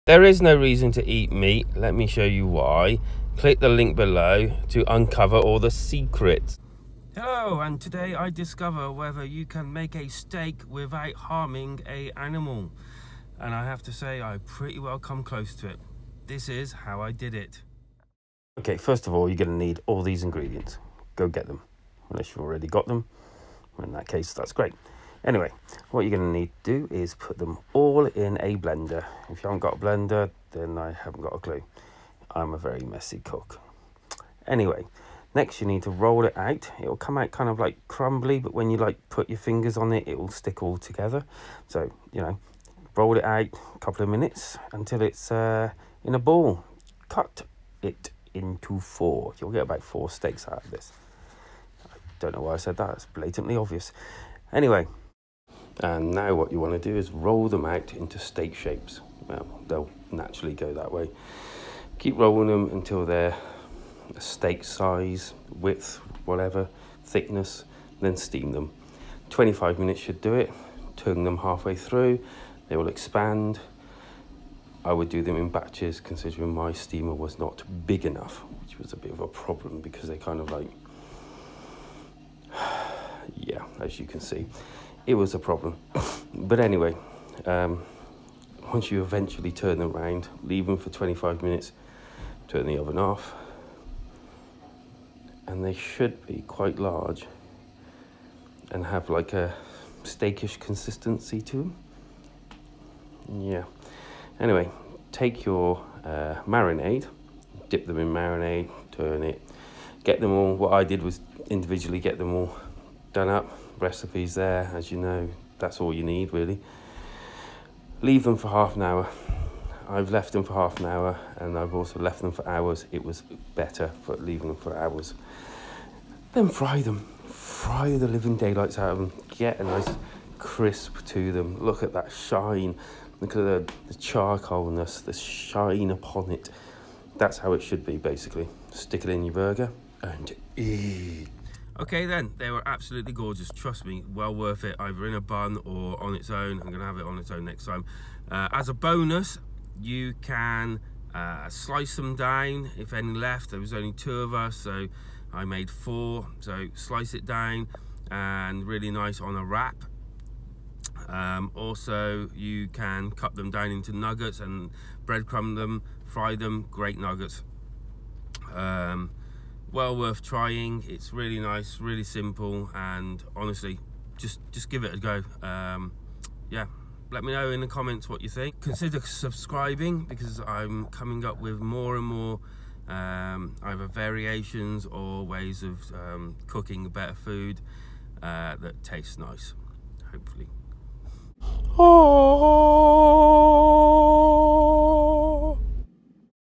I make Vegan Steak so you don't have to eat a cow. Bonus at the end of the video.
It's the Vegan steak in all its glory.

               Ingredients

Vegan steaks or burger

1 1/2 cups vital wheat gluten
1 cup cooked lentils can is better
6 tablespoons water
2 tablespoons nutritious yeast
2 tablespoons tomato paste
2 tablespoons soy sauce
1 teaspoon garlic powder
1/2 tablespoon of paprika smoked
1/4 teaspoon black pepper

vegan steak marinade

1/4 cup water
2 tablespoons olive oil
2 tablespoons soy sauce
1 tablespoon maple syrup